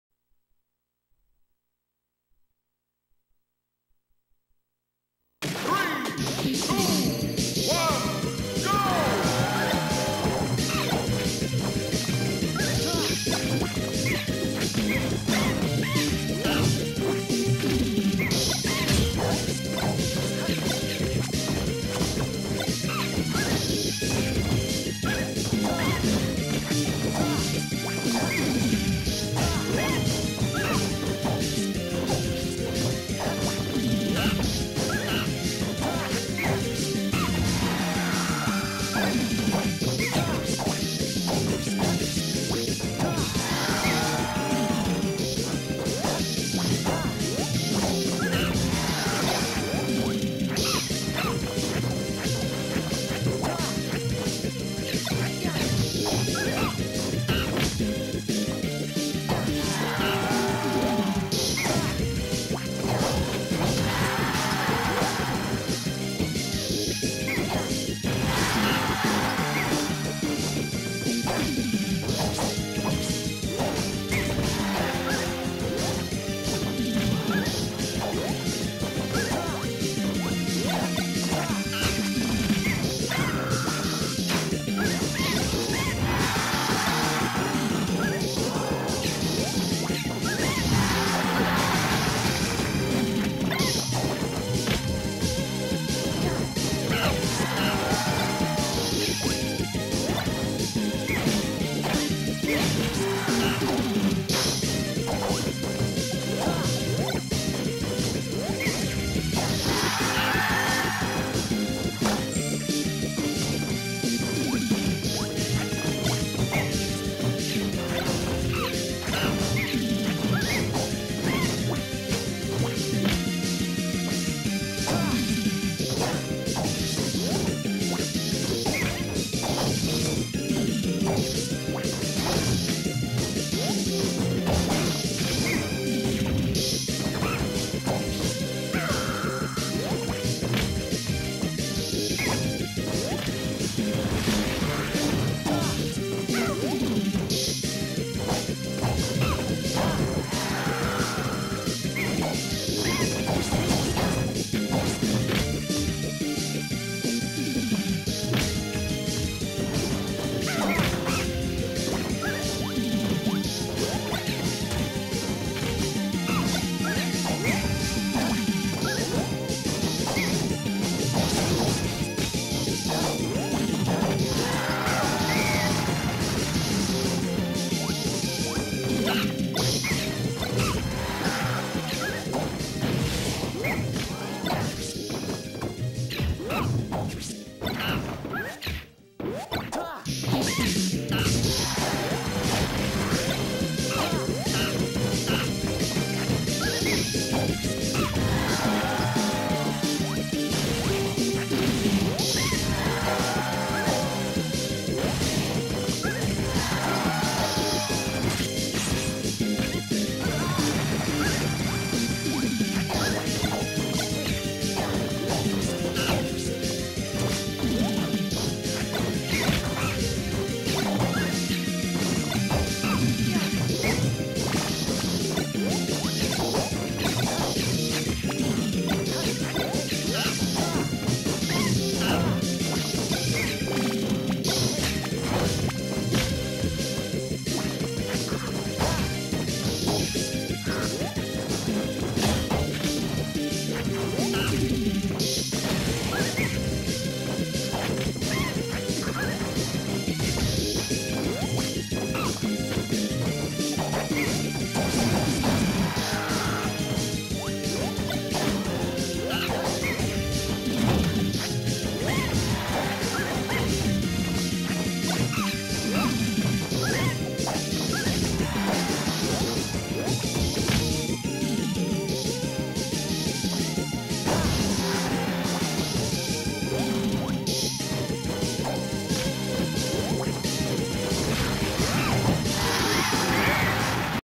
mikeHAZE (Marth) Vs DEHF (Diddy) R1 GF E4J: Koga
Grand Finals at E4J: KOGA

Full results can be found at the link below V